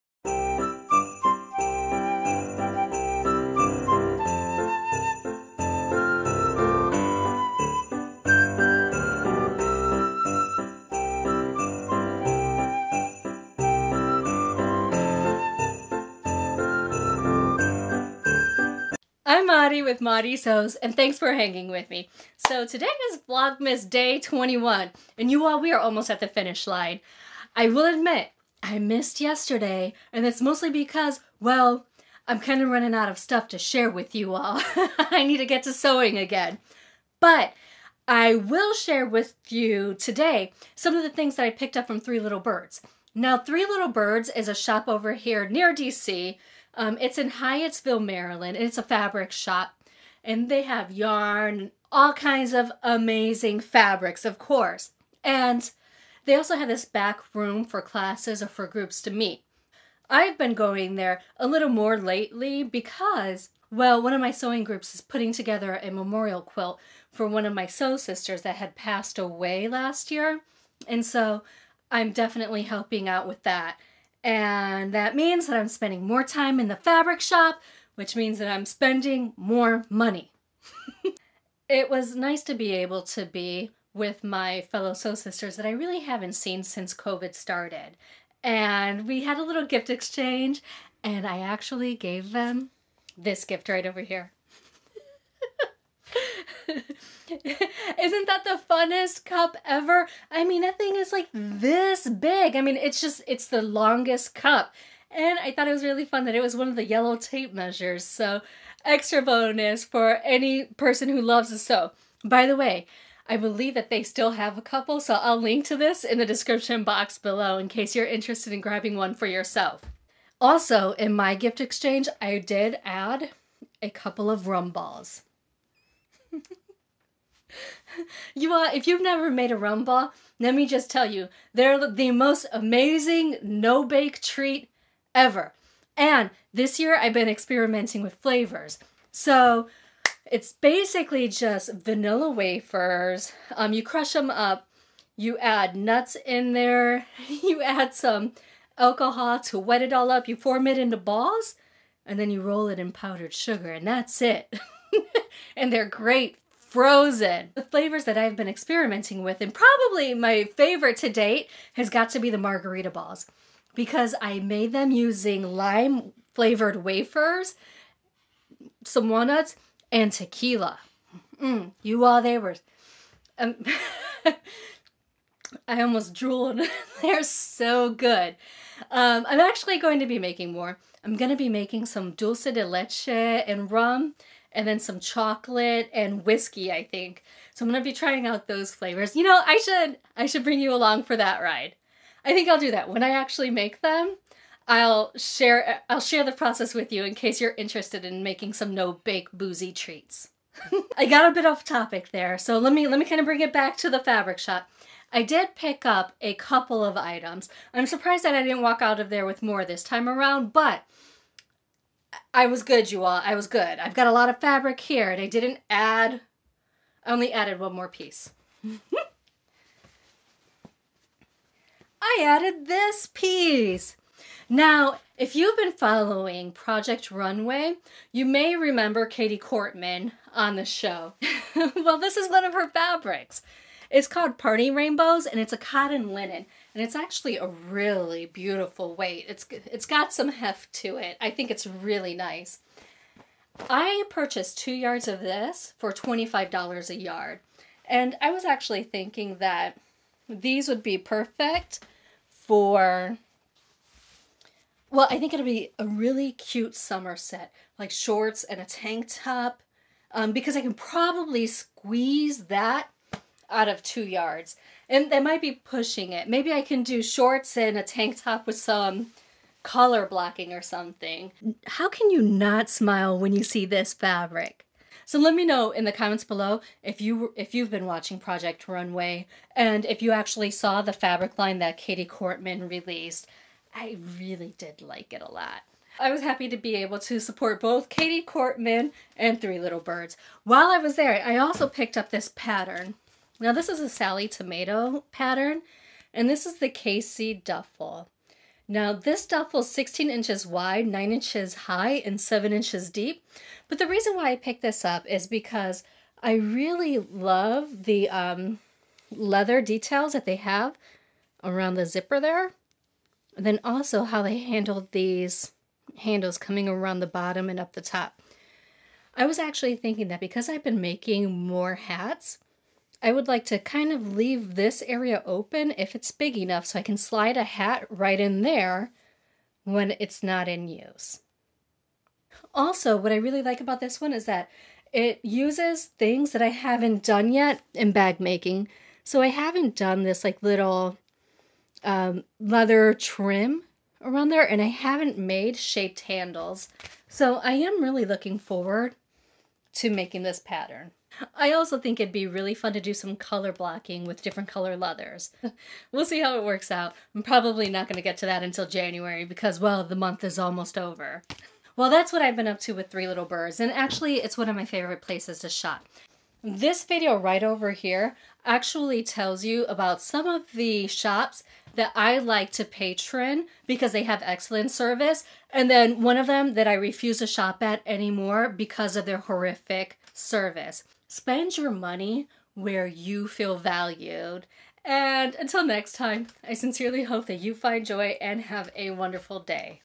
Helping with a Memorial Quilt & Dome Shopping
Helping with a memorial quilt and shopping Katie Kortman’s new fabric

*Mailing Address:*

*If you'd like to contribute towards future videos, supplies, or coffee*